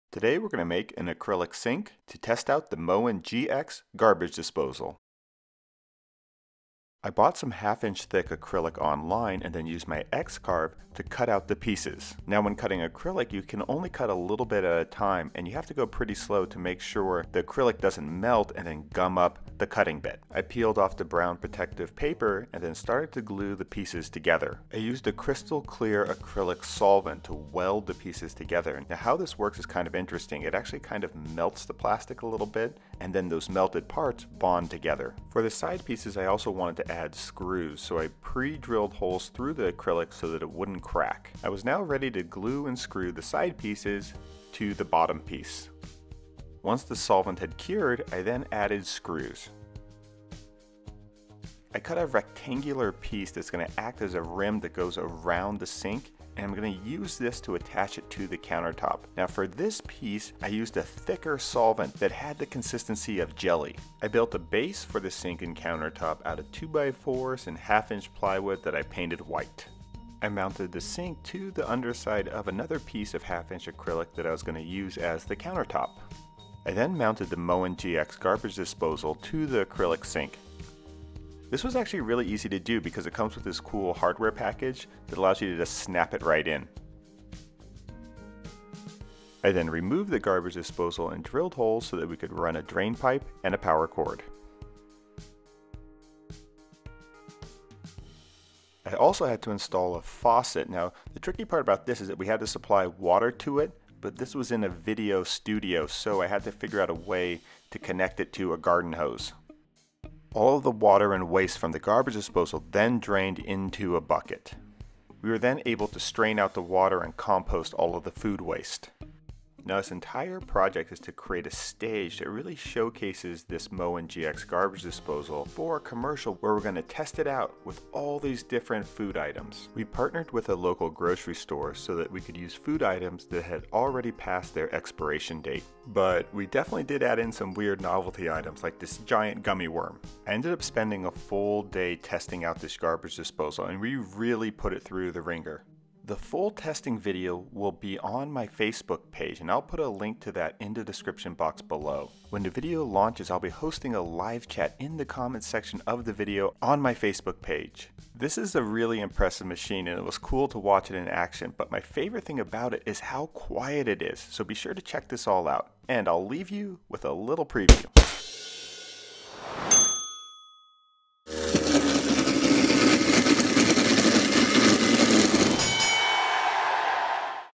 Making an Acrylic Test Sink | for a MOEN GX Garbage Disposal
I built an acrylic sink to test out the MOEN GX Garbage Disposal for a commercial.  The test video will be posted on the HomeMade Modern Facebook page on May 1st, 7pm EDT.
I will be hosting a live chat in the comment section of that video post when it goes live.